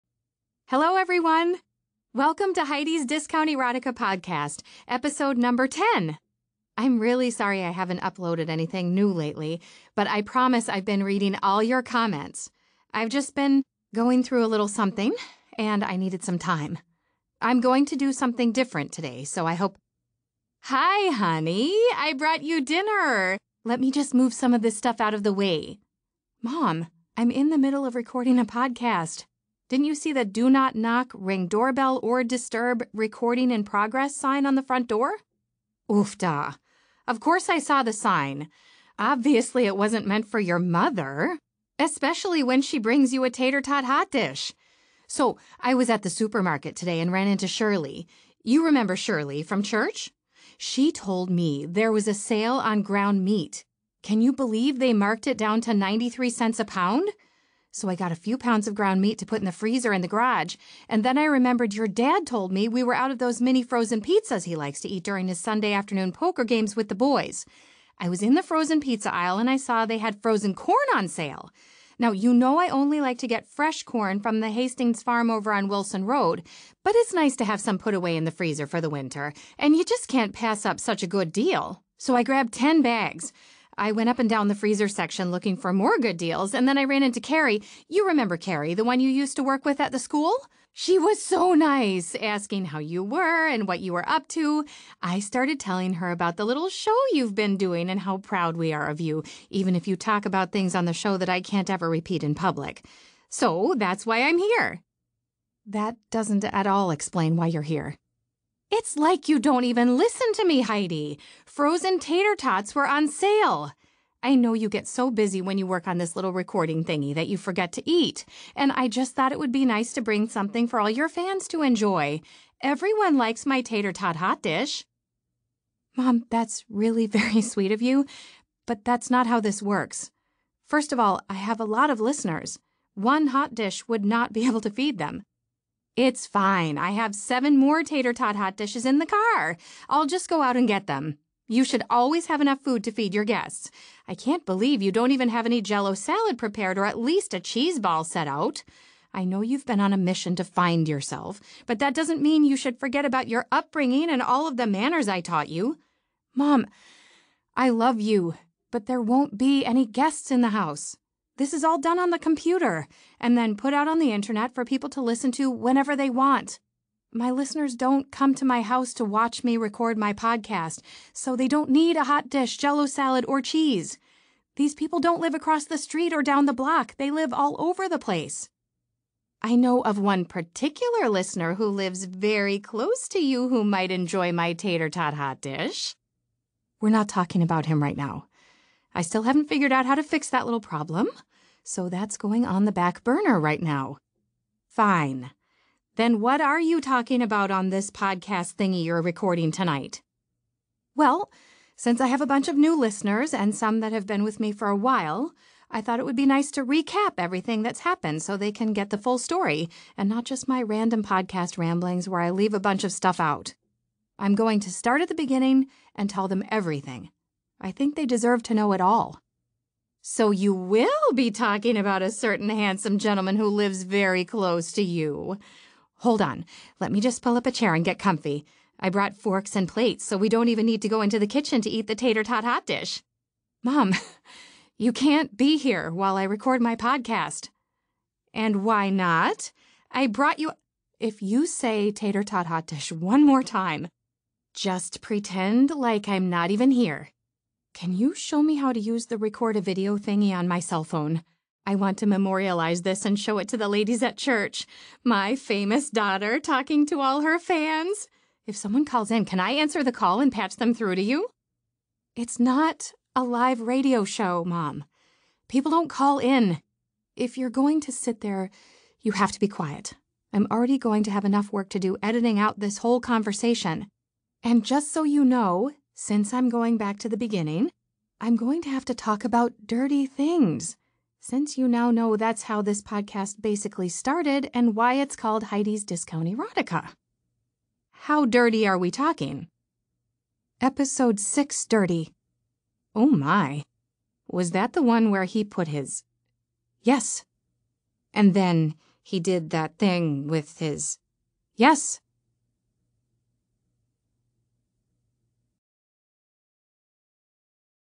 Heidi's Guide to Four Letter Words (Audiobook) by Tara Sivec, Andi Arndt
Best Romance AudioBooks
Heidi's Guide to Four Letter Words (Audiobook) by Tara Sivec, Andi Arndt Reviews:
Publisher's Summary
Cowritten by USA Today best-selling author Tara Sivec and award-winning narrator Andi Arndt, a hysterically funny, heartfelt romance - created specifically for the audio format!
Nothing good ever comes from drinking a box of wine alone. So when I decided to entertain my drunken self by setting up some hand-me-down podcasting equipment and reading the steamy parts from romance novels, I never thought anyone would actually listen. The fact that I admitted my huge crush on my sexy next door neighbor made the whole thing even more mortifying. But sometimes life surprises you, and that’s how my podcast, Heidi’s Discount Erotica, was born.
Now I, Heidi Larsen, a sweet former kindergarten teacher in Waconia, Minnesota, lead a scandalous double life reading erotic novels to the listening world. And with each episode, I find myself embracing my new alter ego more and more. Now I’m starting to feel more comfortable in my own skin and do things I never would have dreamed of - like kissing my neighbor.
Look out, Waconia, because Heidi’s on the loose! She’s in your ears, in your hearts, and down your pants...wait, that didn’t sound as good as it did in my head. Well, you get the picture, don’tcha know!